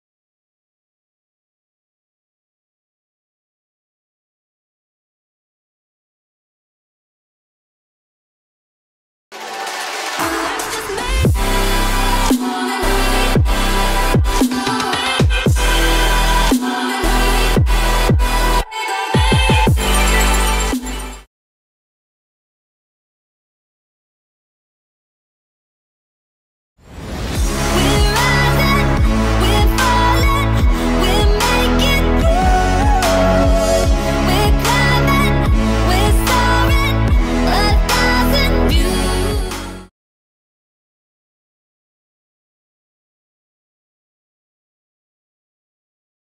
Pick my new intro!
Hey everyone sorry the video was so short but I want to know which intro y’all like better the 1st one or the 2nd one comment which one u like best bye!